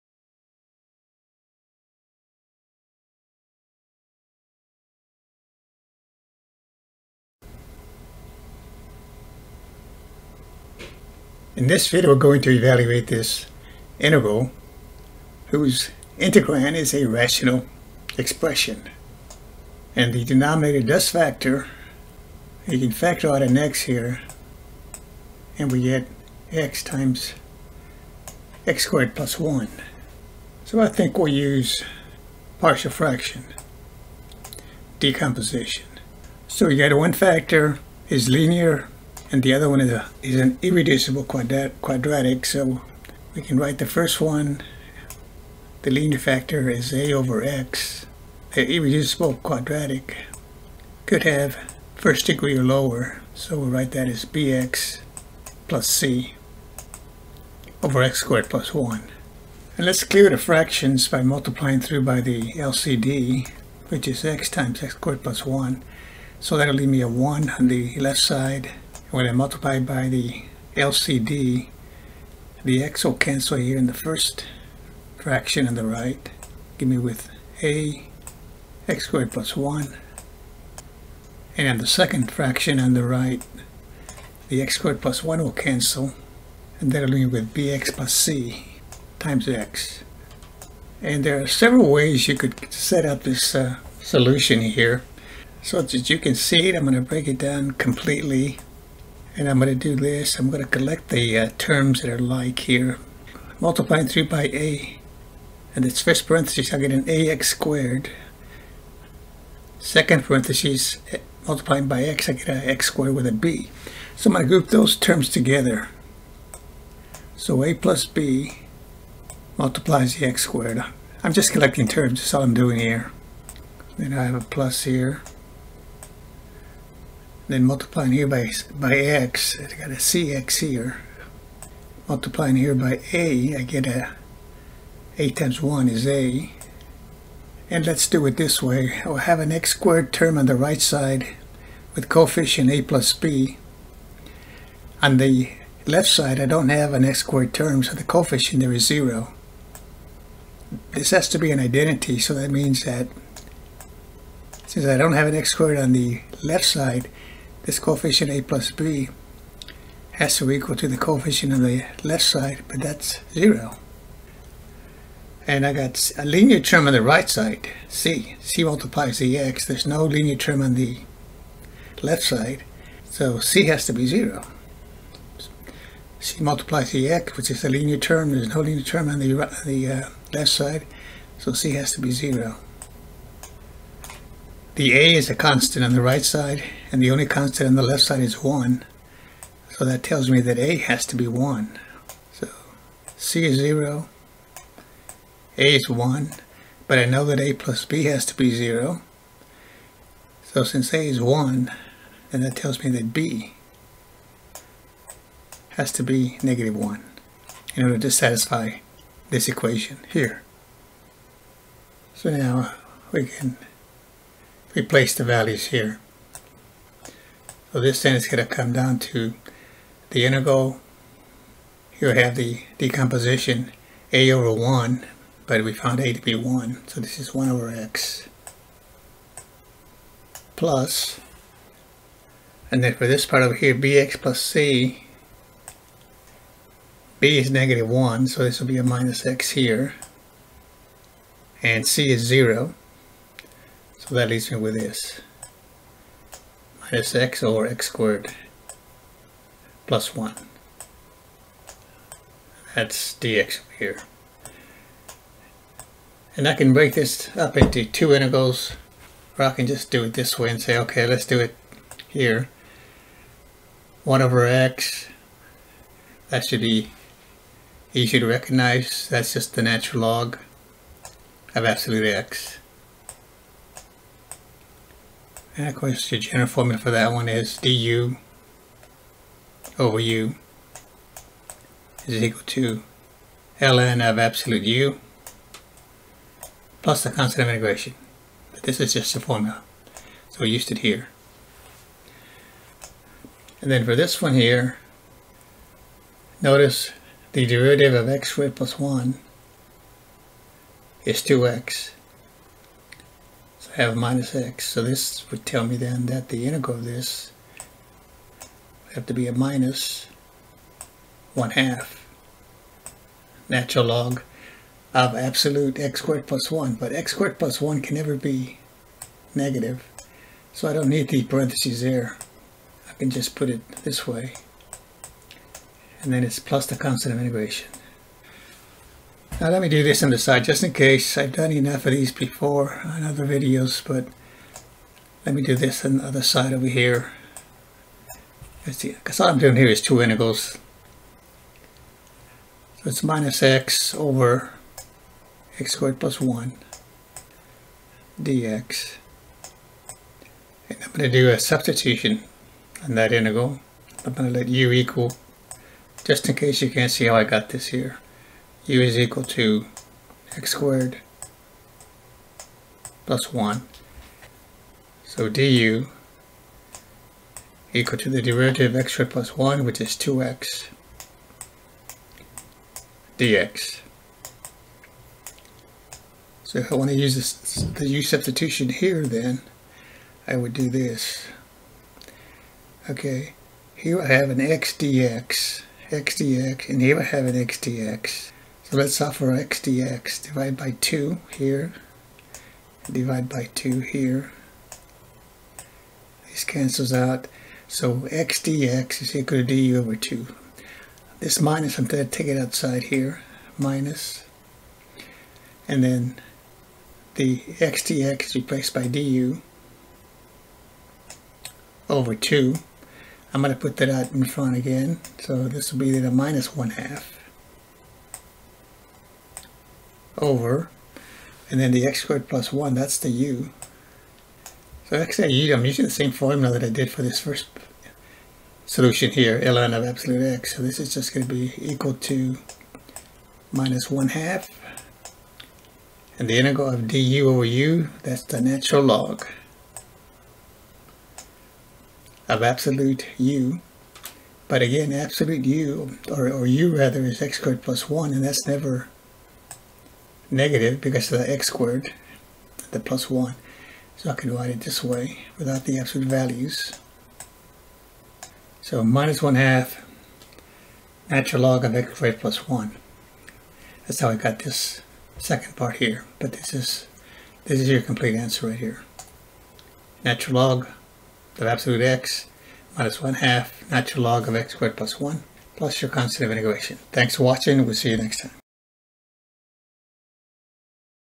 The Integral of 1/(x^3 + x)
In this video, we evaluate an integral using partial fraction decomposition.